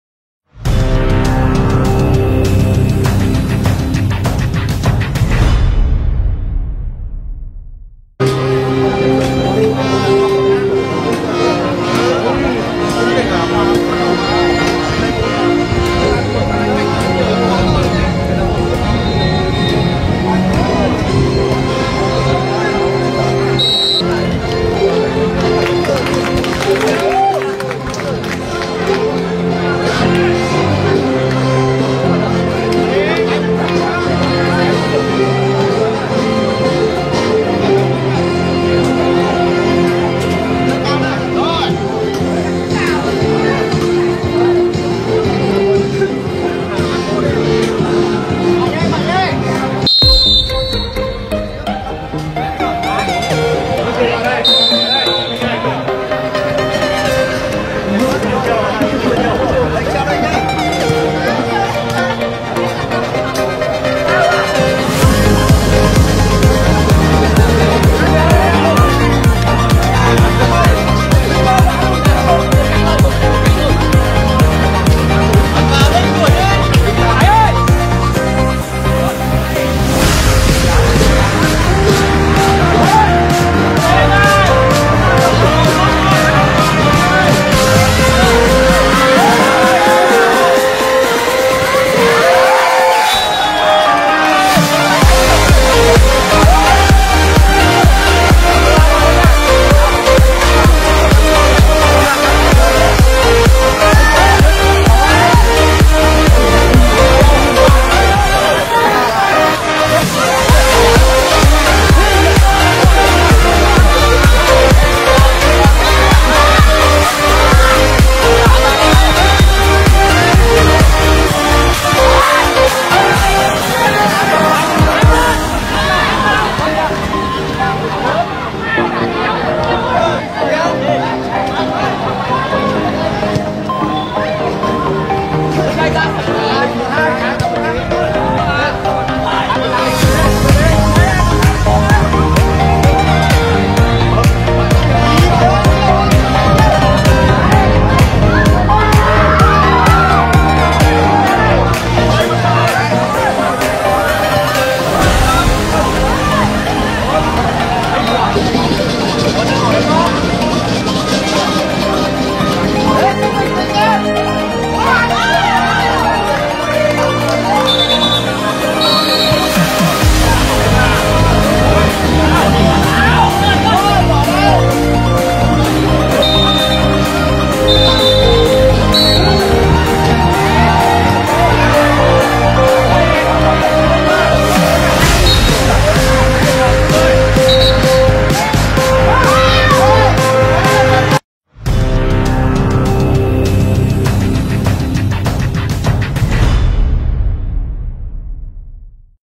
🎬 Highlights: LÊ QUÝ ĐÔN -vs- YÊN HÒA🏆 Vòng 5 - GIẢI BÓNG ĐÁ 93 - 96 CUP KẾT NỐI
🎬 Highlights: LÊ QUÝ ĐÔN  -vs- YÊN HÒA
🏆 Vòng 5 - GIẢI BÓNG ĐÁ 93 - 96 CUP KẾT NỐI
📍 Sân Bóng PKKQ - Lê Trọng Tấn - Hà Nội